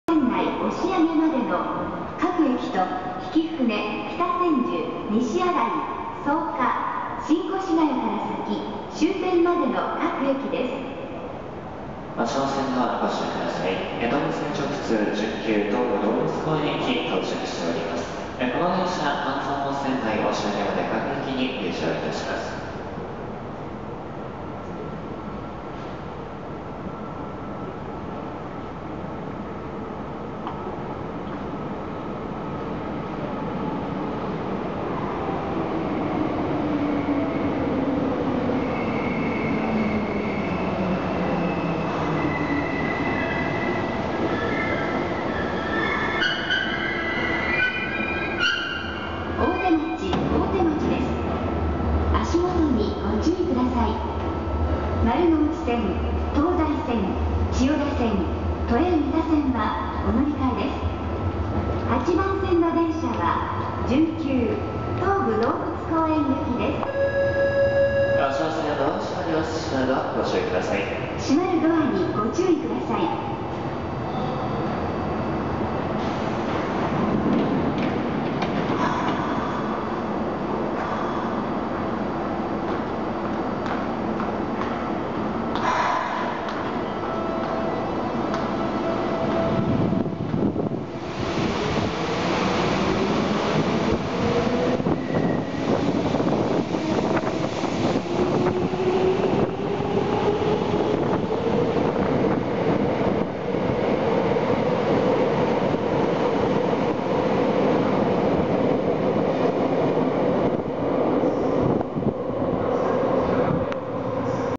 8000系半蔵門線 準急 東武動物公園行き 大手町駅を発着
東京メトロ 半蔵門線 大手町駅で
住吉 錦糸町 北千住方面
準急 東武動物公園行き8000系を撮影しました